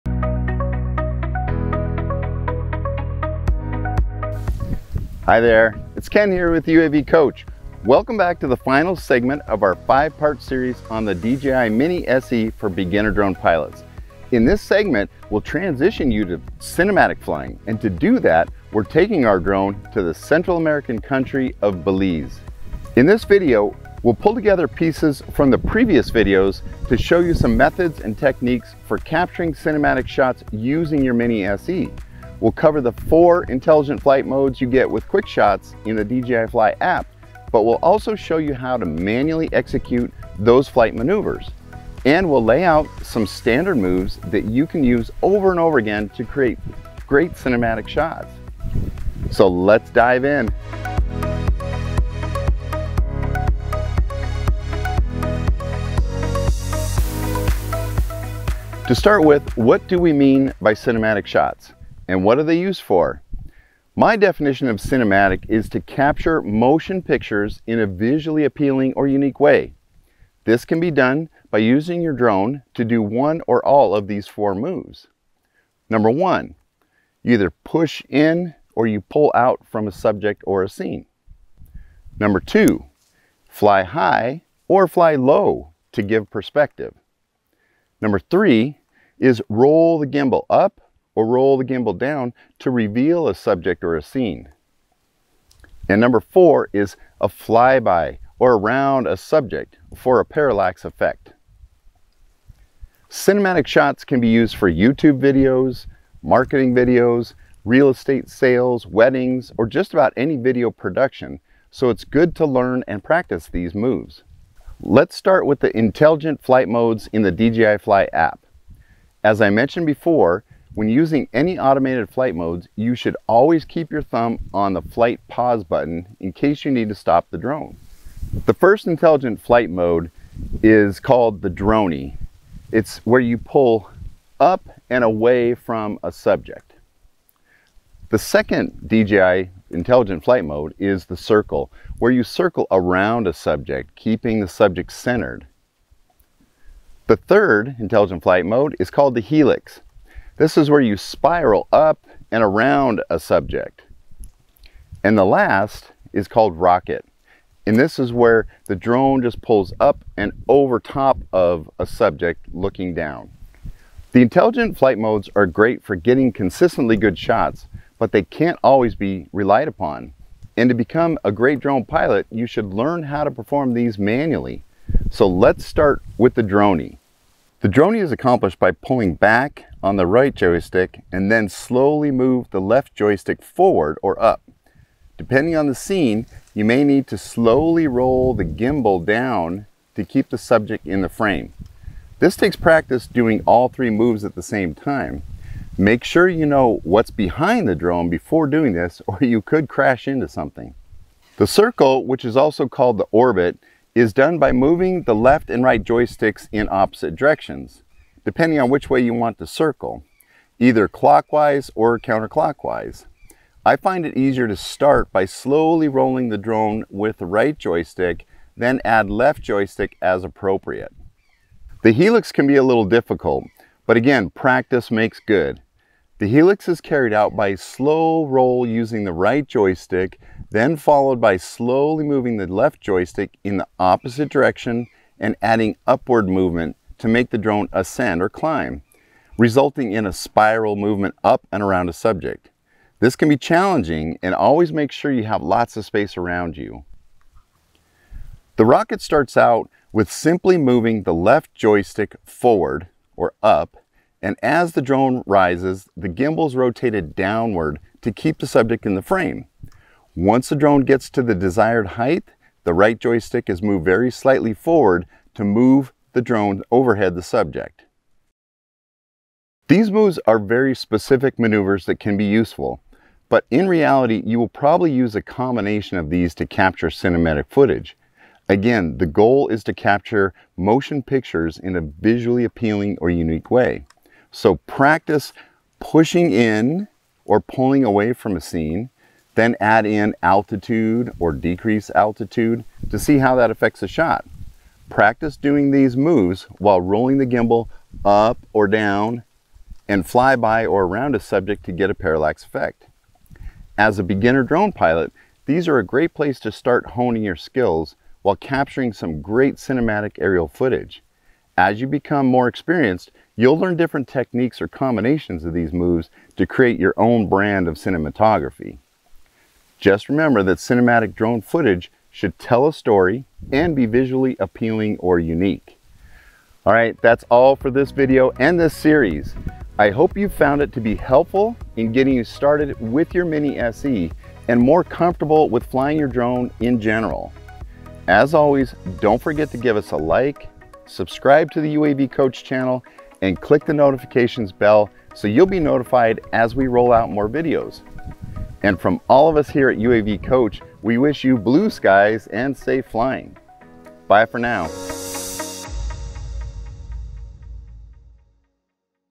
DJI Mini SE Beginner's Guide | Cinematic Flying
In this video, we'll show you how to get those beautiful cinematic shots you've always dreamed of capturing.

🛒 Mavic Mini SE (Amazon)
🛒 Mavic Mini Batteries (Amazon)
🛒 Mavic Mini Charging Hub (Amazon)

0:00​ B-Roll Footage & Introduction
1:00 What are Cinematic Shots?
1:56 Intelligent Flight Modes
2:52 Performing Flight Modes Manually
2:56 "The Dronie" - Manual
3:21 "The Circle" - Manual
3:42 "The Helix" - Manual
4:08 "The Rocket" - Manual